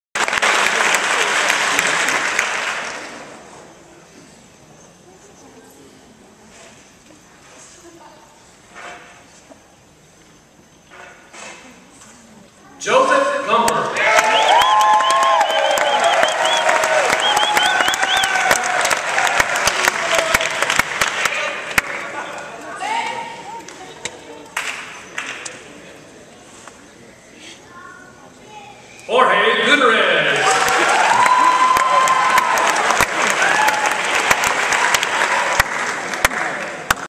Video 15
Summerlin Academy Medal Ceremony  2011
Cadets receive Summerlin diplomas and medallions
Joe Gumpper receives diploma